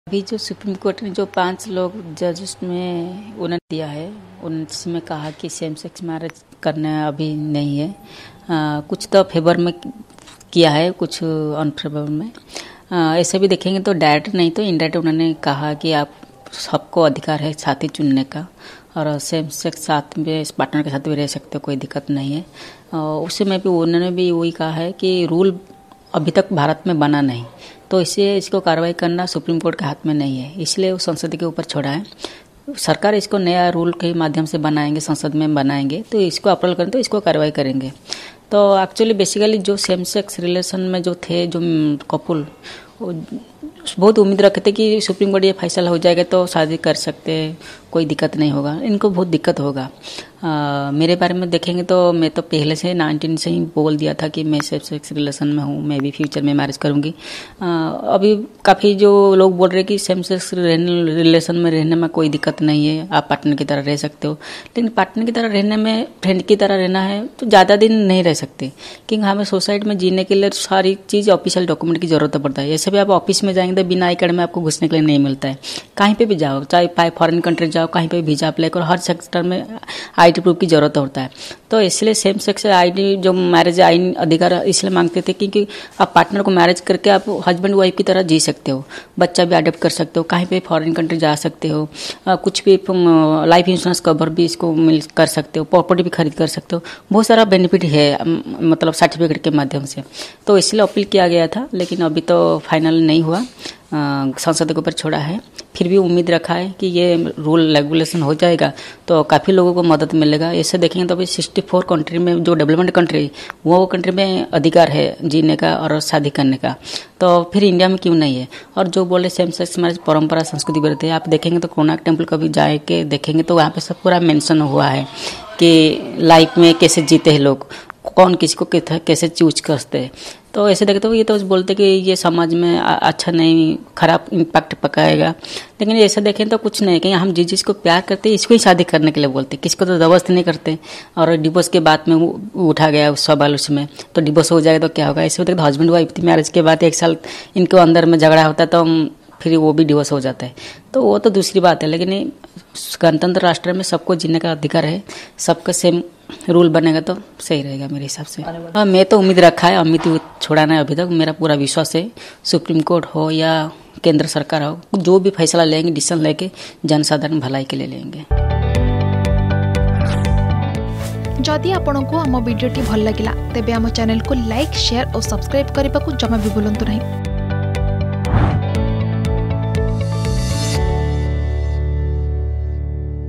'ସମଲିଙ୍ଗୀ ବିବାହ' ଉପରେ ଦୂତୀ ଚାନ୍ଦଙ୍କ ପ୍ରତିକ୍ରିୟା | Supreme Court Verdict
ସମସ୍ତଙ୍କୁ ନିଜ ଜୀବନସାଥୀ ବାଛିବାର ଅଧିକାର ଅଛି : ଦୂତୀ ଚାନ୍ଦ